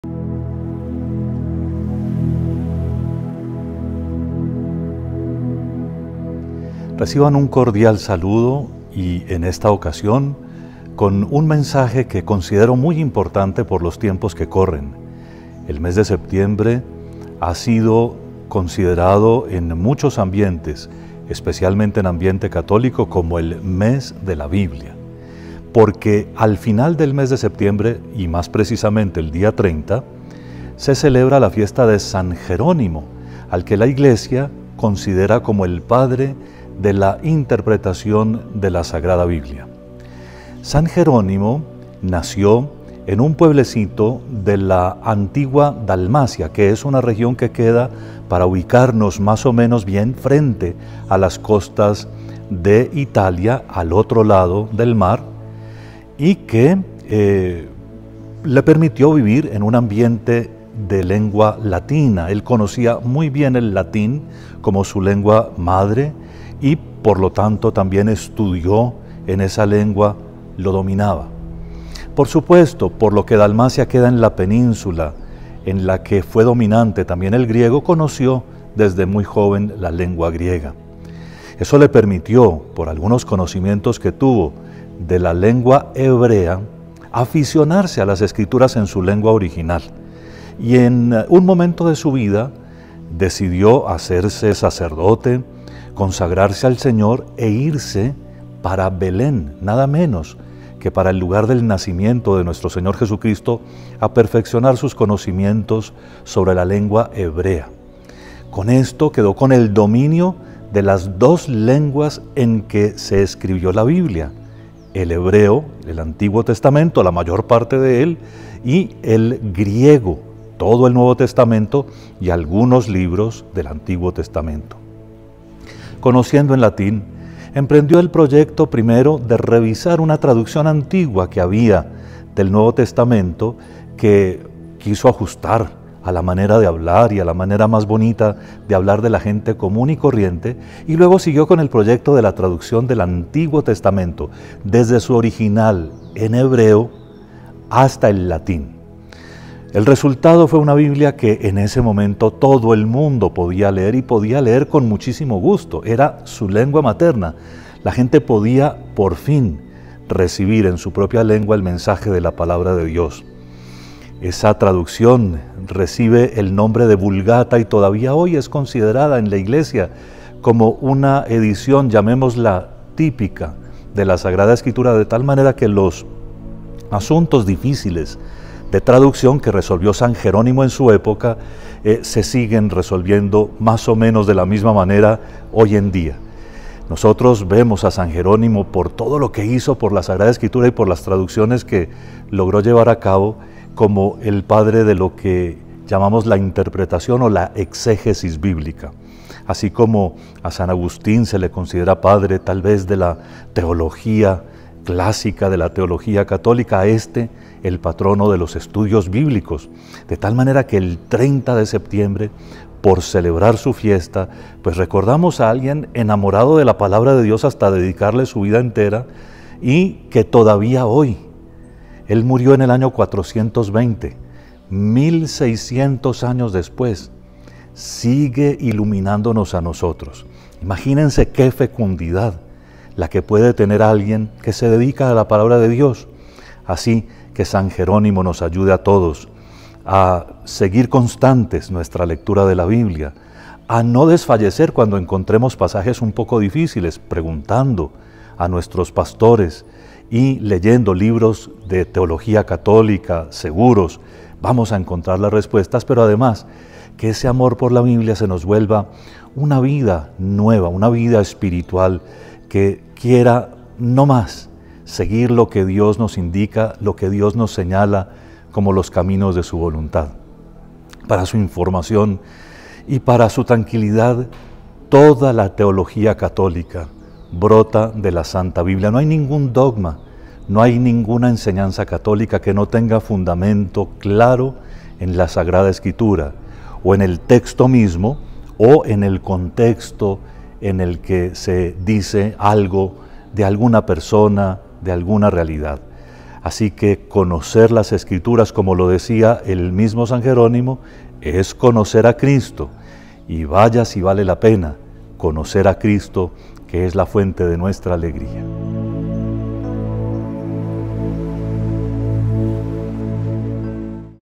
¿QUIÉN FUÉ SAN JERÓNIMO? Monseñor José Miguel Gómez Rodríguez, Arquidiócesis de Manizales.
En este video, el Arzobispo de Manizales nos presenta la vida y legado de San Jerónimo, el gran estudioso y traductor de la Biblia. Conocido por su obra monumental, la Vulgata, San Jerónimo dedicó su vida a la comprensión profunda de las Escrituras y su traducción al latín. Acompáñanos en este recorrido por la historia de un hombre cuya fe y dedicación transformaron la manera en que el mundo cristiano accede a la Palabra de Dios.